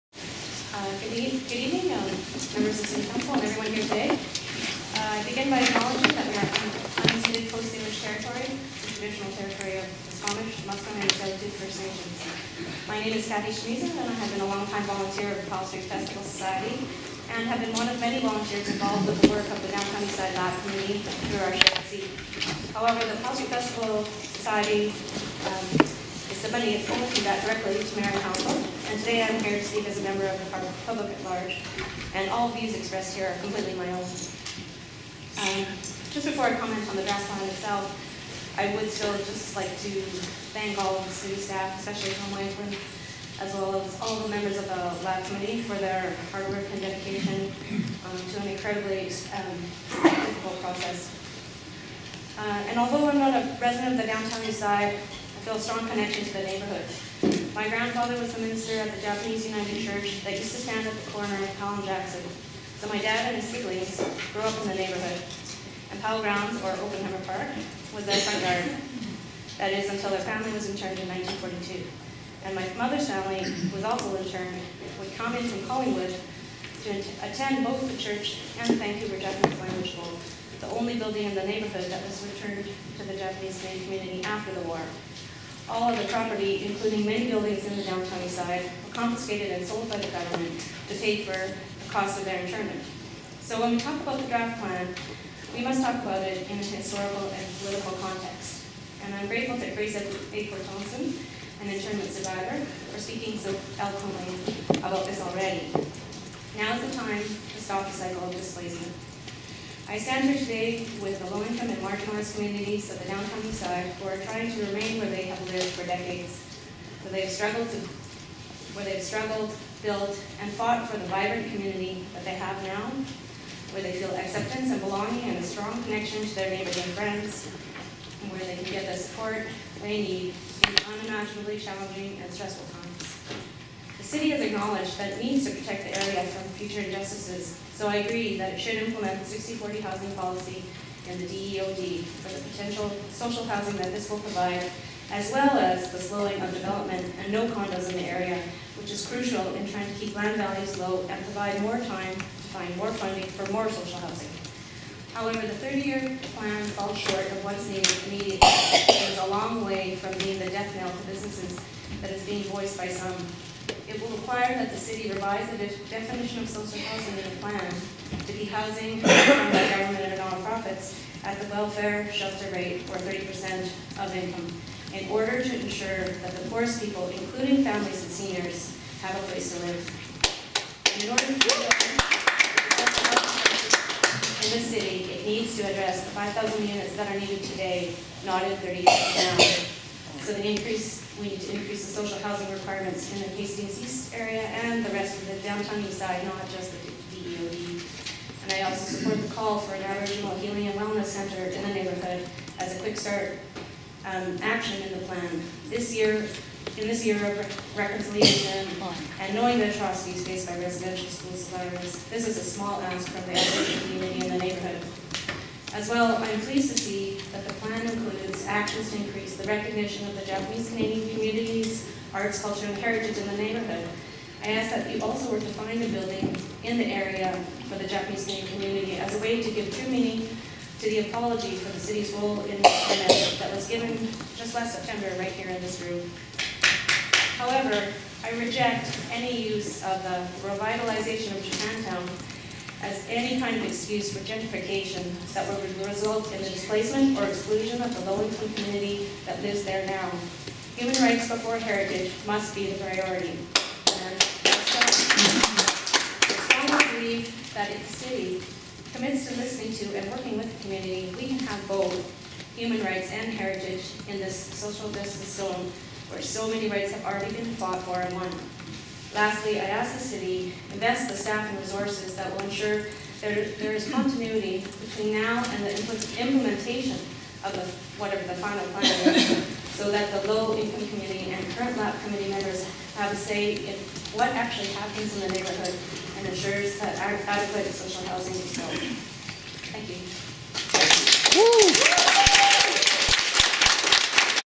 Kathy Shimizu speaks on DTES Local Area Plan
Kathy Shimizu delivers a speech on the Downtown Eastside Local Area Plan before Vancouver City Council. A total of 142 speakers were signed up to speak and the Council Committee meeting spanned 3 days. The majority of Council voted in favour of the plan on March 15, 2014. Further details on CityHallWatch.ca